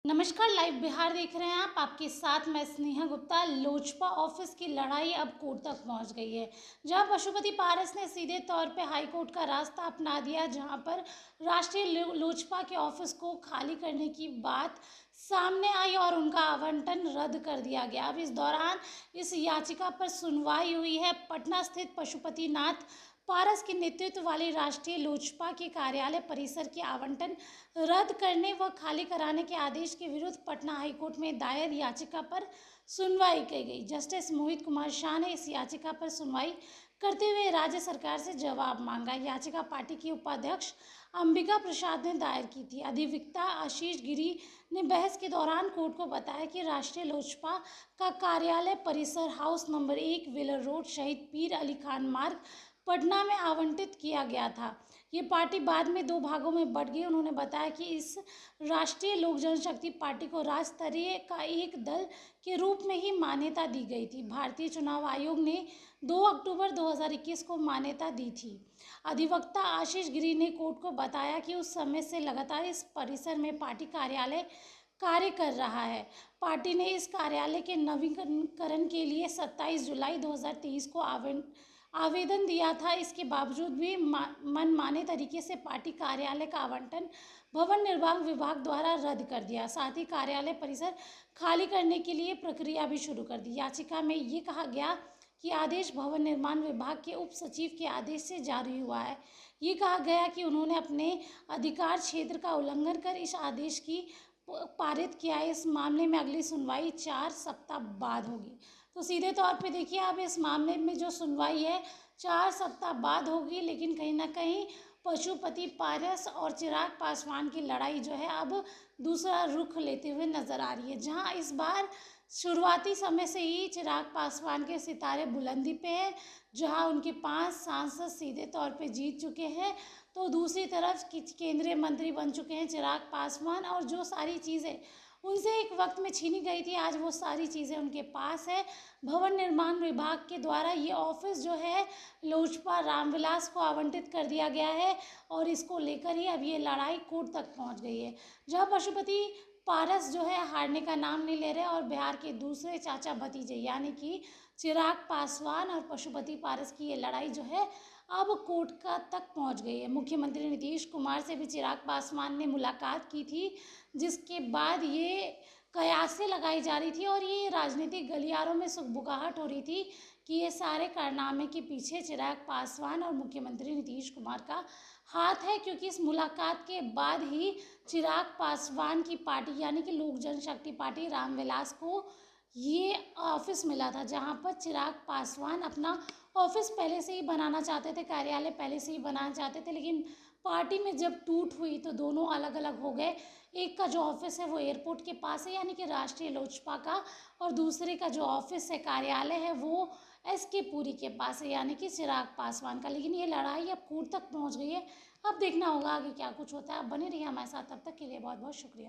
Pashupati Paras के कार्यालय मामले में सुनवाई, सरकार को देना है जवाब, High Court ने साफ कर...
Live Bihar Digital Network